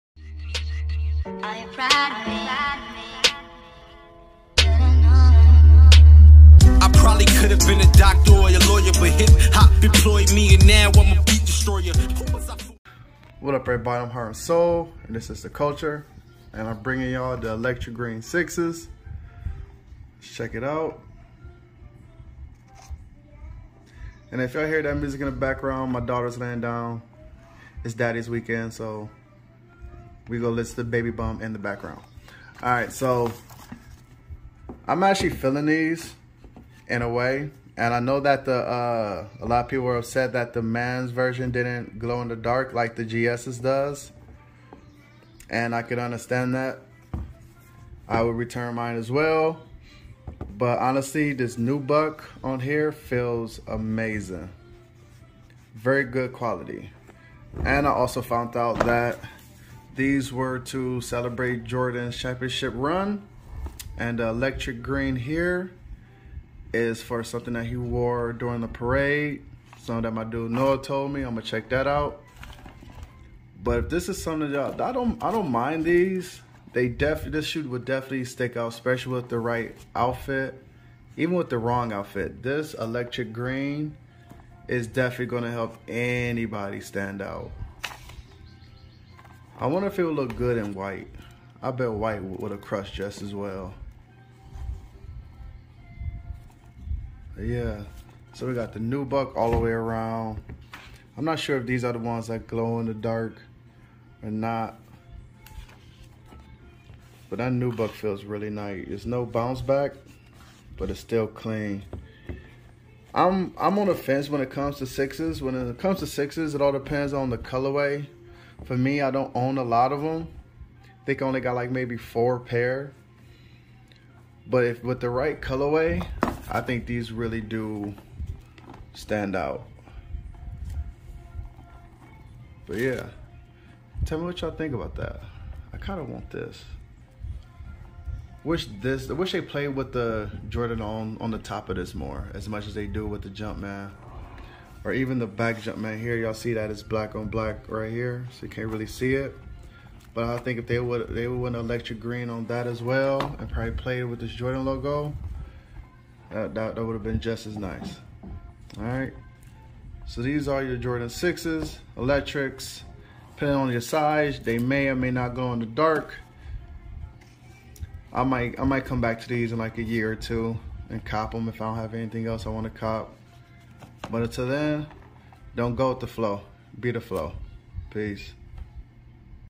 Electronic green jordan 6
looking at the electronic jordan 6 in hand